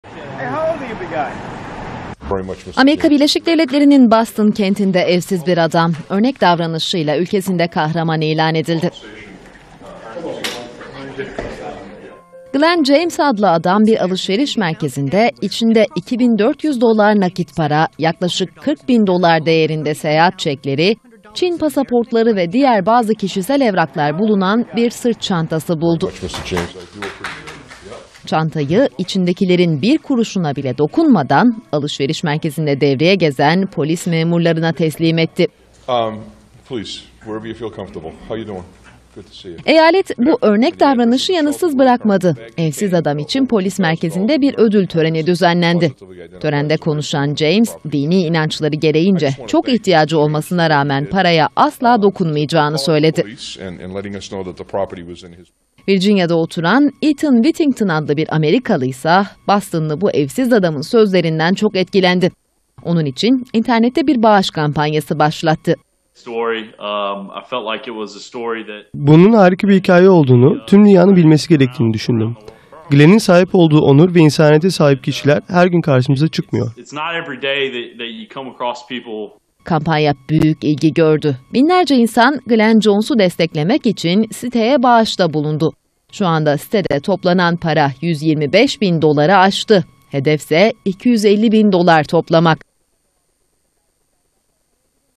Evsiz kahraman
ABD'nin Boston eyaletinde evsiz bir adam kahraman oldu. İçinde 42 bin dolardan fazla para olan bir sırt çantası bulan adam onu yetkililere teslim etti. Hikayesinden etkilen yüzlerce kişinin bağışları sayesindeyse şimdi 125 bin dolar var